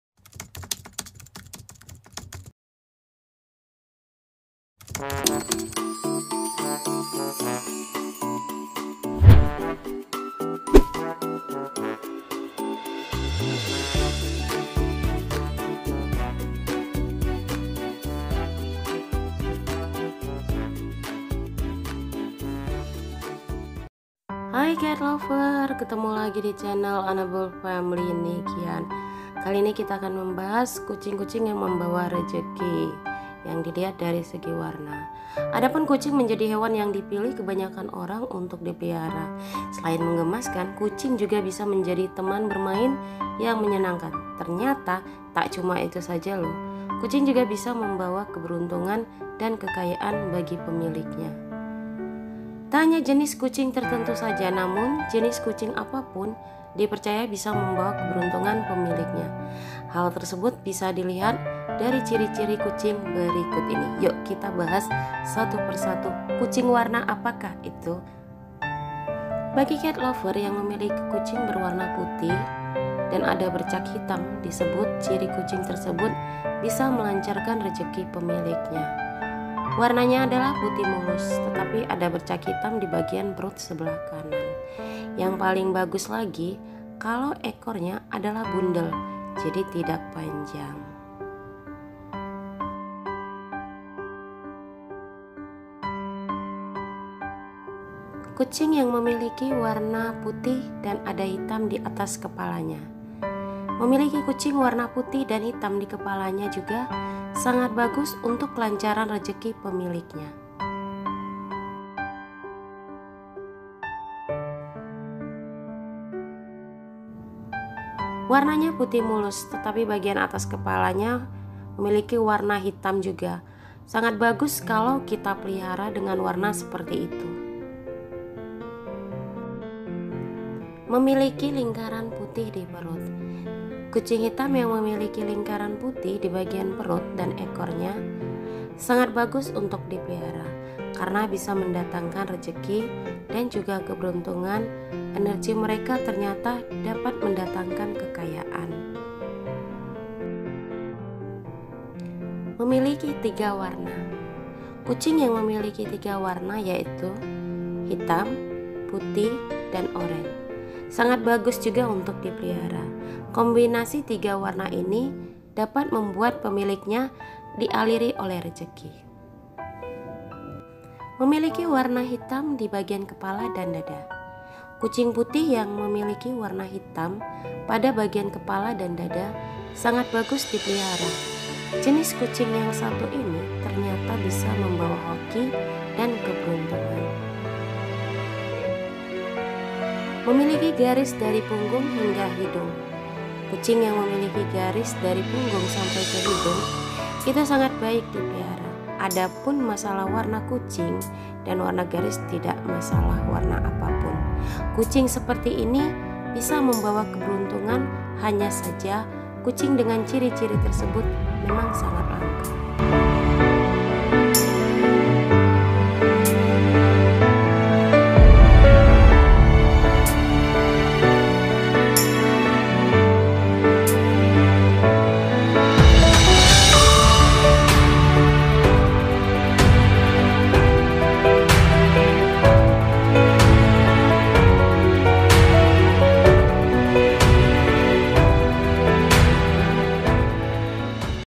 Ciri - ciri warna Kucing yang membawa rezeki & keberuntungan terhadap pemiliknya
Hai Catlovers🐈😻..pengen tahukan kucing warna apa saja yang bisa membawa keberuntungan terhadap pemiliknya.